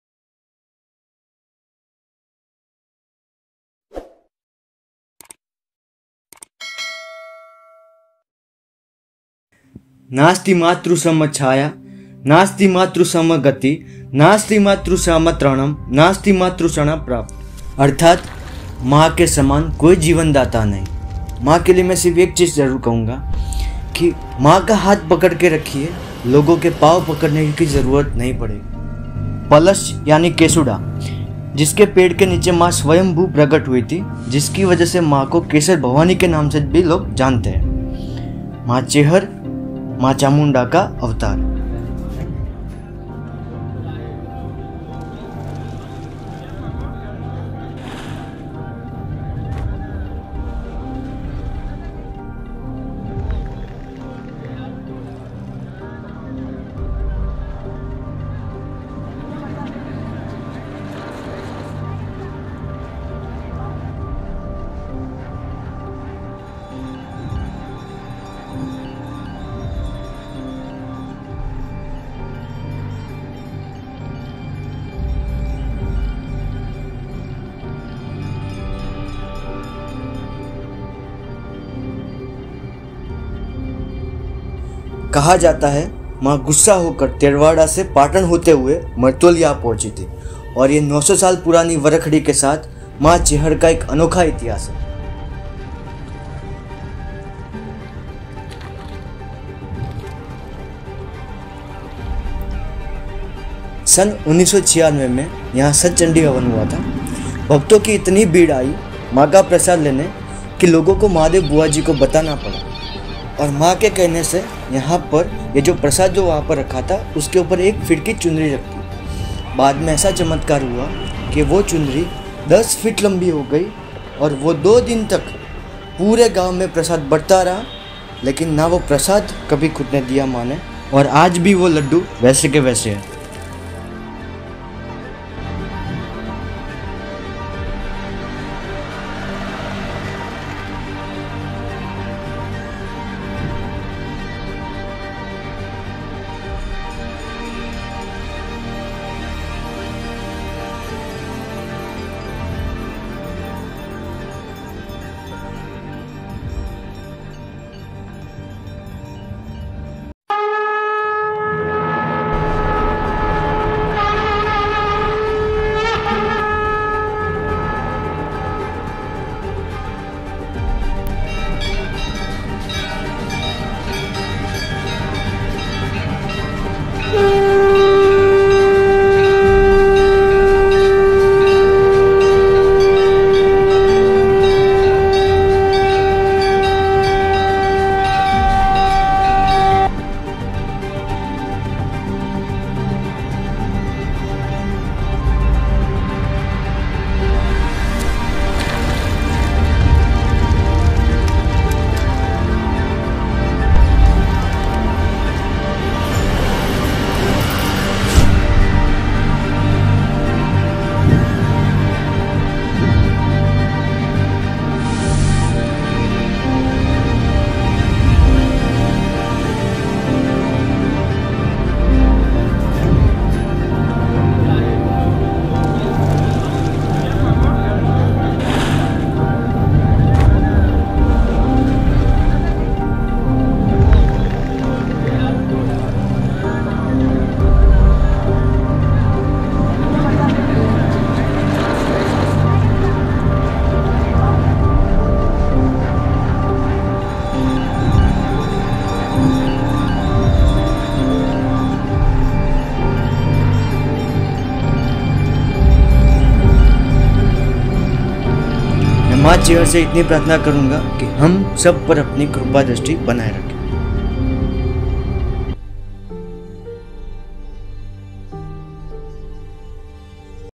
CHEHAR MAA MARTOLI ︳ચેહર માતા (મરતોલી ︳Kesar Bhavani Temple
IN THIS VIDEO WE SHOOT A VIDEO OF CHEHAR MAA( MARTOLI).
CHEHAR MAA MARTOLI#KesarBhavaniTemple#BHARGAVSONY#

DOP: DHRUVIT  GUPTA

:::: MARTOLI ::::

Chehar Maa's main temple is in Martoli. It's a small Village close to Mehsana, Gujarat, India. It's a very sacred, very beautiful, and famous spiritual place. A large number of Chehar Maa devotees visit this place every day.

The temple is sanctified with the 900-year-old sacred tree where the mother transformed into the Sukshma Sharira. A beautiful silver carving of the mother is also placed under this tree.

CHEHAR MAA MARTOLI#KesarBhavaniTemple#BHARGAVSONY#